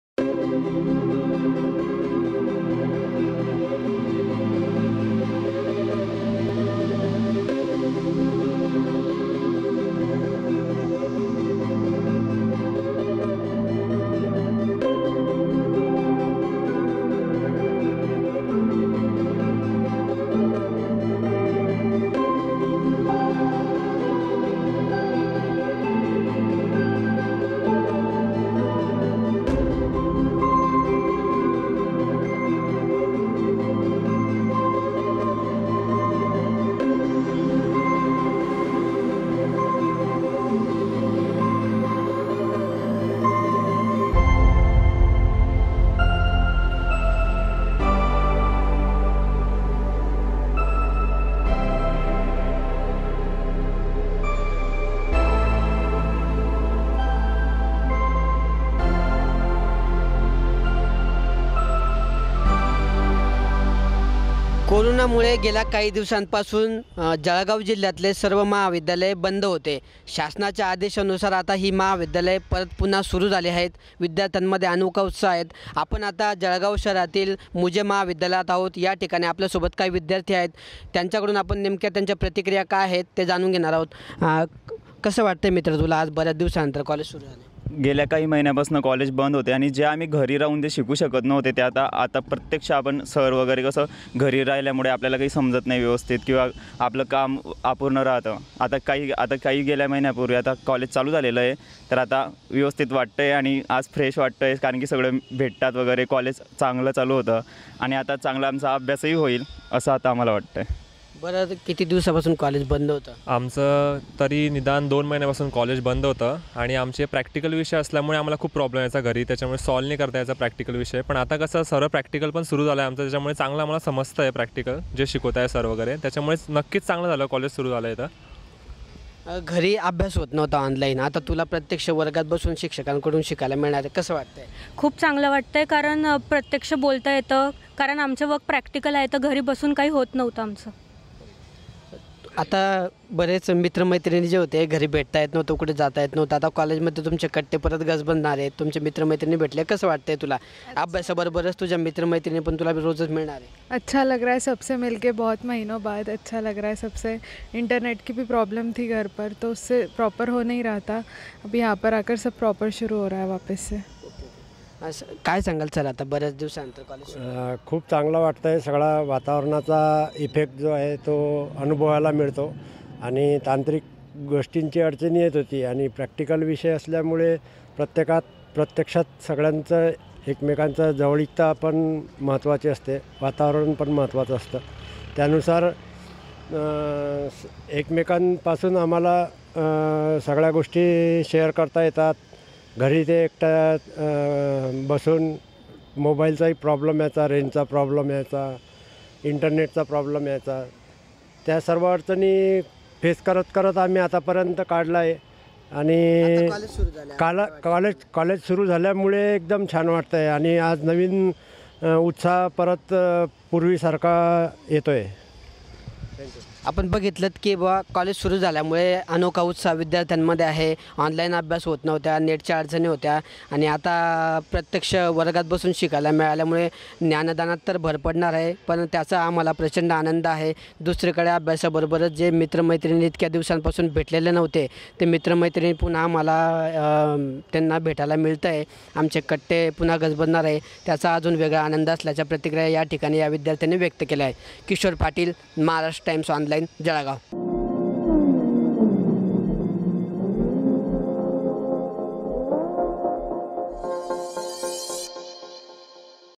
Jalgaon | महाविद्यालये पुन्हा सुरु; प्रात्यक्षिक परीक्षाही होणार ऑफलाईन
आजपासून राज्यातील महाविद्यालय आणि विद्यापीठं पुन्हा सुरु झाली आहेत. अनेक दिवसांनंतर जळगावातही महाविद्यालये पुन्हा गजबजले आहेत.महाविद्यालय सुरू झाल्याने विद्यार्थ्यांमध्ये प्रचंड उत्साह आणि आनंदाचे वातावरण आहे.
ऑनलाईन ऐवजी आता थेट वर्गात बसून पुन्हा शिकायला मिळणार असल्याने विद्यार्थ्यांमध्ये उत्साह आहे..मित्र मैत्रिणींसोबत पहिल्याच दिवशी महाविद्यालयातील कट्टे गजबजलेले दिसले.